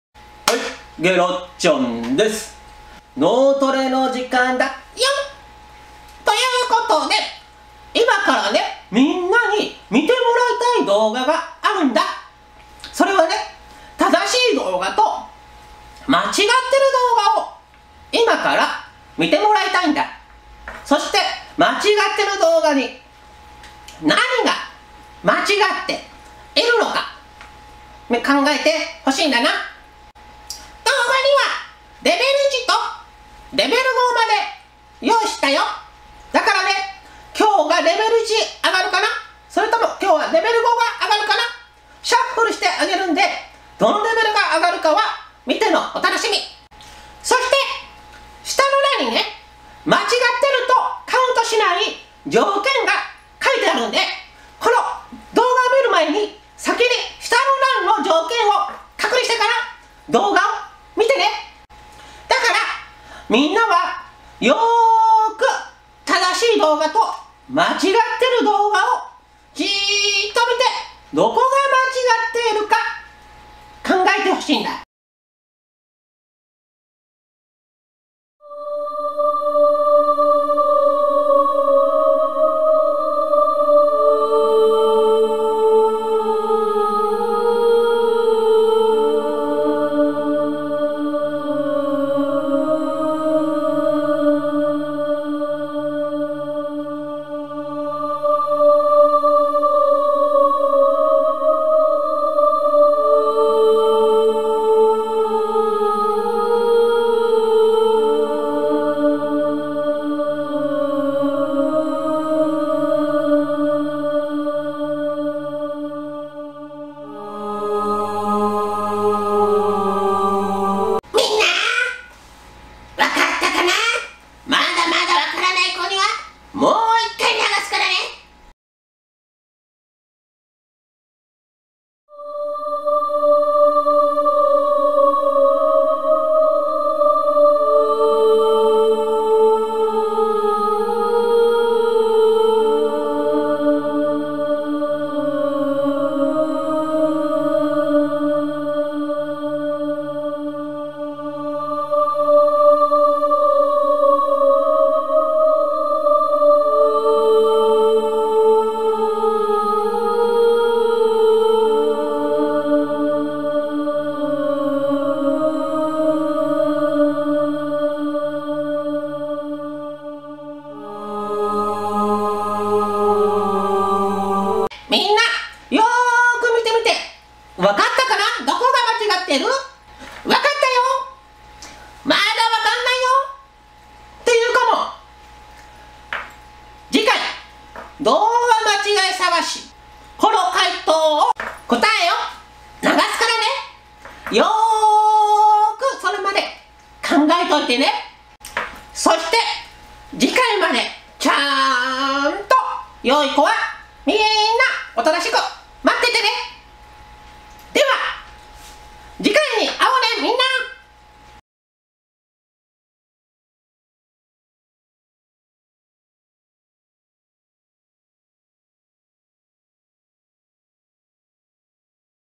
動画間違い探し 脳トレの時間 ２
間違いは、１か所です。同じ間違いは１カウントのみです。
間違いとカウントしない条件は、
風の動き、カメラの動きはカウントしません。